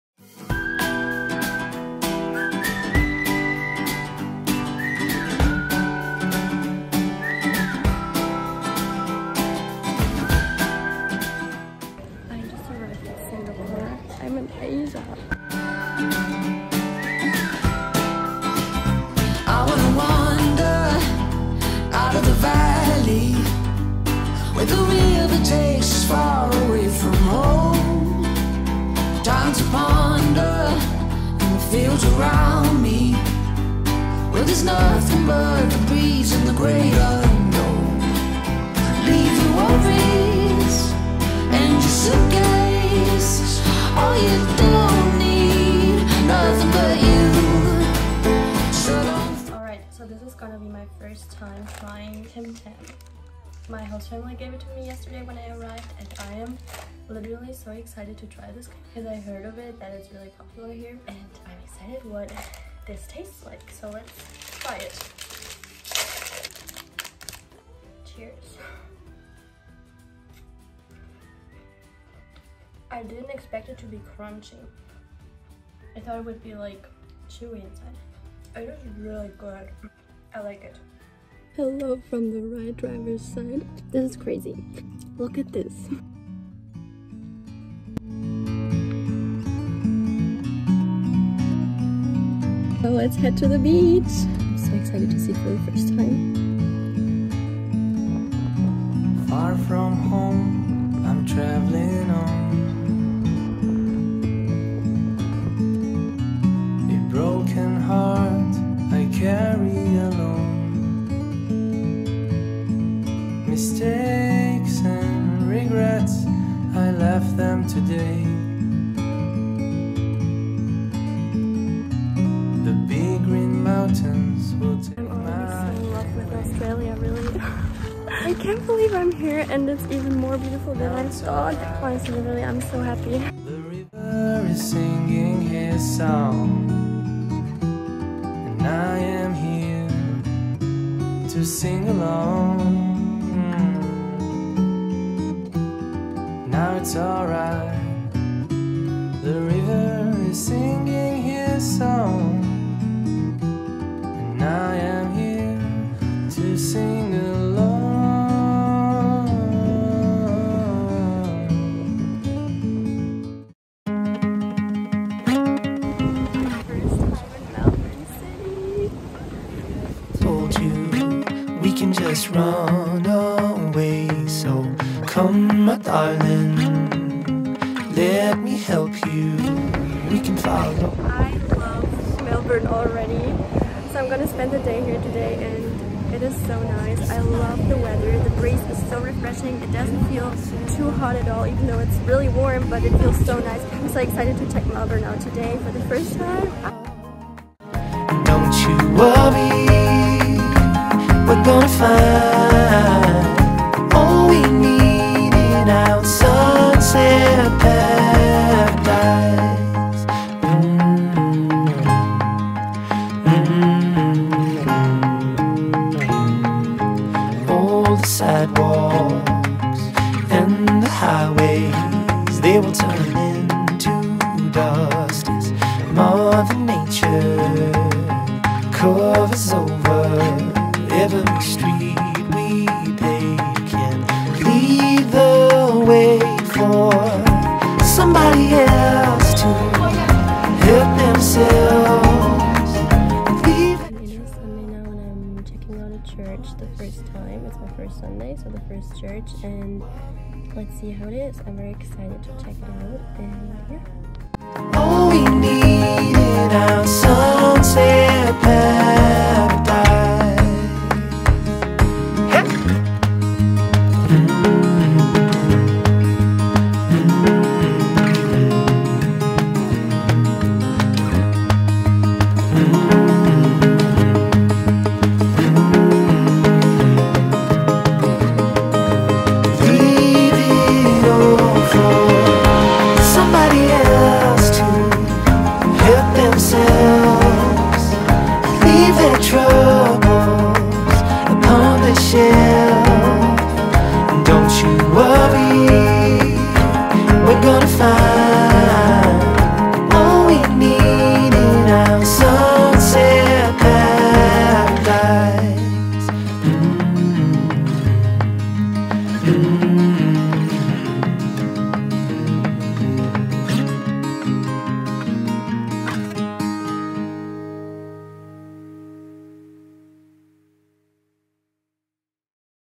My first week in Australia/flight/beach love/exploring Melbourne/trying Tim Tam-ANNIKA
Hello mates!
I am finally here! In this video I am showing you some snippets of my first week in Australia. It's been very busy and a lot the last days so I didn't get to film everything but I still caught some major moments. I hope y´all enjoy those little insights of my new home here.
Leave any questions in the comments.
Stay blessed,
Annika